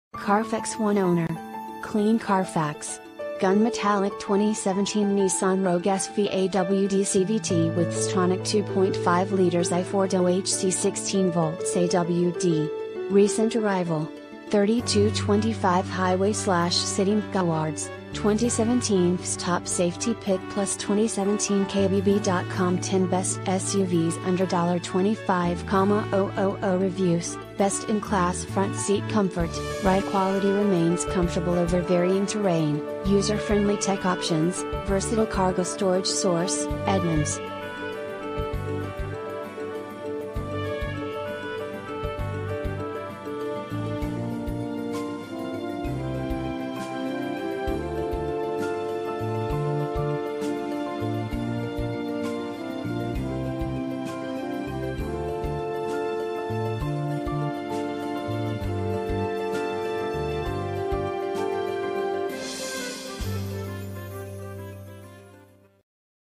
2017 Nissan Rogue Denison Sherman Durant N530093P
CARFAX One-Owner. Clean CARFAX. Gun Metallic 2017 Nissan Rogue SV AWD CVT with Xtronic 2.5L I4 DOHC 16V AWD.brbrRecent Arrival! 32/25 Highway/City MPGbrbrAwards:br  * 2017 IIHS Top Safety Pick+   * 2017 KBB.com 10 Best SUVs Under $25,000brbrbrReviews:brbr  * Best-in-class front seat comfort; ride quality remains comfortable over varying terrain; user-friendly tech options; versatile cargo storage Source: Edmunds